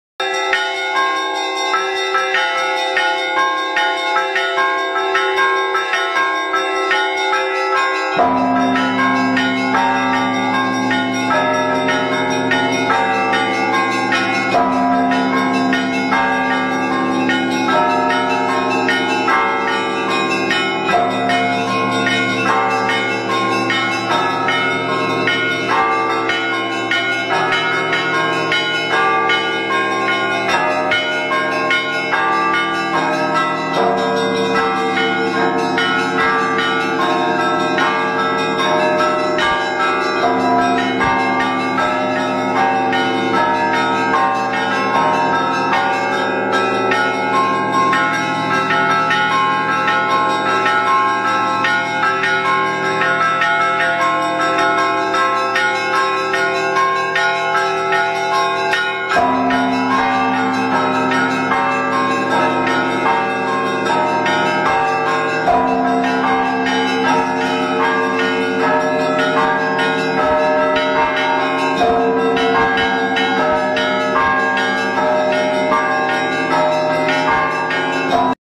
Αγιορείτικη Κωδωνοκρουσία
Aγιορείτικες  ,Αναστάσιμες καμπάνες
Πανηγυρικές καμπάνες από Αγιορείτικη Μονή. Το περιβόλι της Παναγίας δοξολογεί τον Θεό με ήχους μελωδικούς.